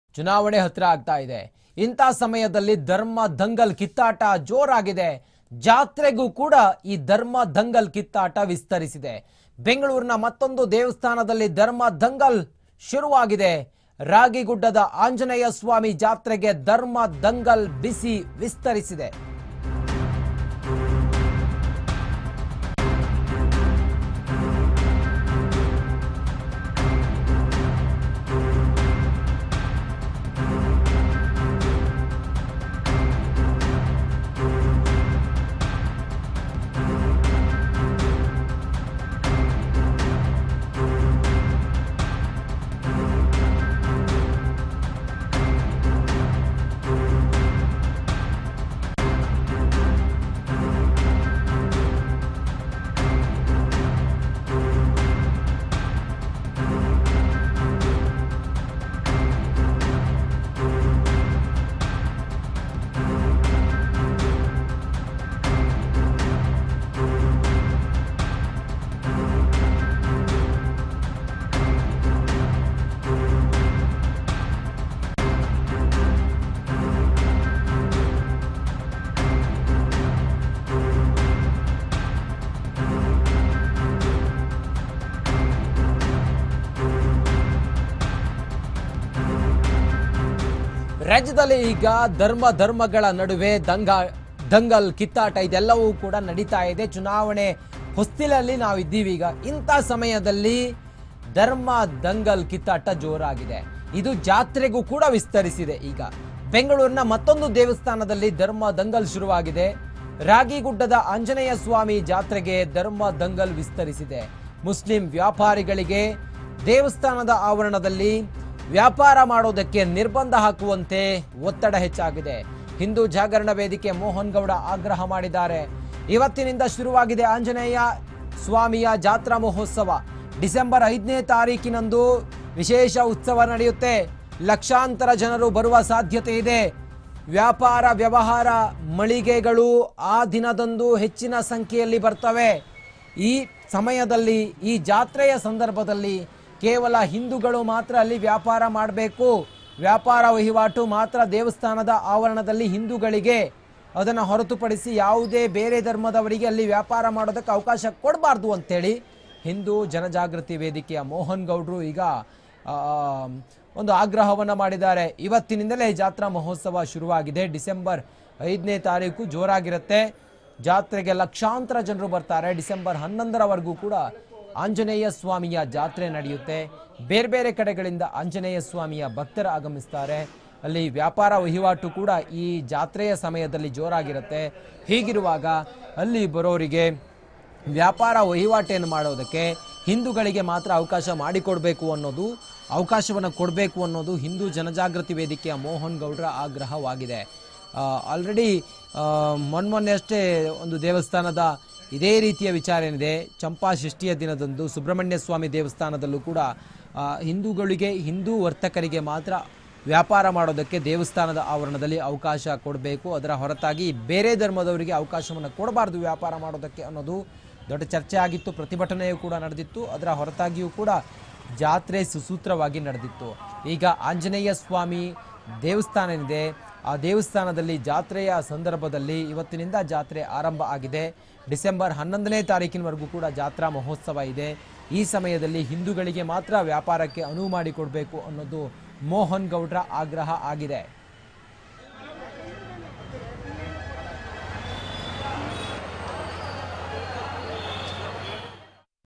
ಜಾತ್ರೆಗೂ ವಿಸ್ತರಿಸಿದ ಧರ್ಮ ದಂಗಲ್​ ಕಿತ್ತಾಟ.. ಬೆಂಗಳೂರಿನ ಮತ್ತೊಂದು ದೇವಸ್ಥಾನದಲ್ಲಿ ಧರ್ಮ ದಂಗಲ್​​!
Btv News Kannada: ಜಾತ್ರೆಗೂ ವಿಸ್ತರಿಸಿದ ಧರ್ಮ ದಂಗಲ್​ ಕಿತ್ತಾಟ.. ಬೆಂಗಳೂರಿನ ಮತ್ತೊಂದು ದೇವಸ್ಥಾನದಲ್ಲಿ ಧರ್ಮ ದಂಗಲ್​​!